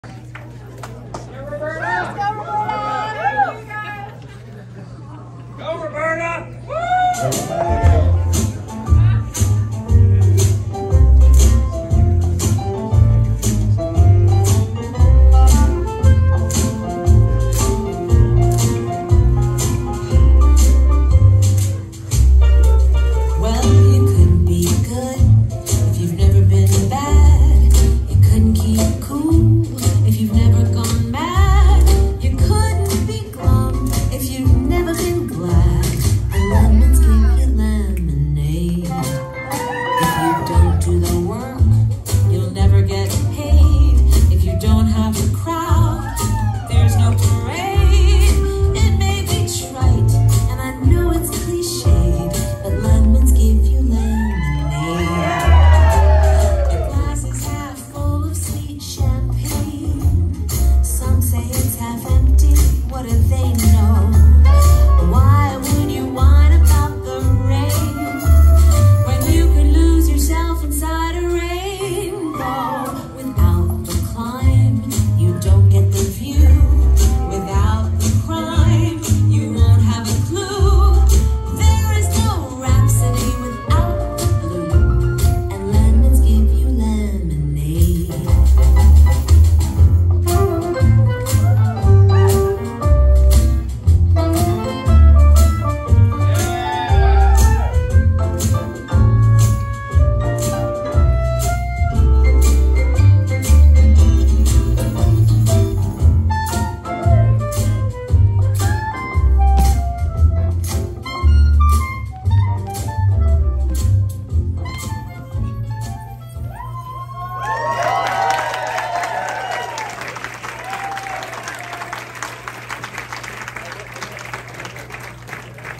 4th Gala Showcase - Roberta Pearlstein & Hugo Miguez - West Coast Swing Improv
Welcome to the 4th Annual Gala Showcase where students of District Dance Academy get to show off their skills! Enjoy our playlist of 25 performances across all different styles.